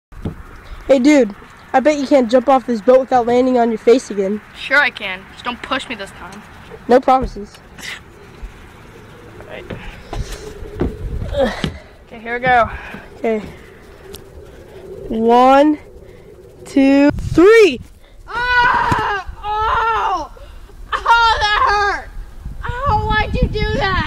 EPIC Boat FAIL!
Person falls off boat lol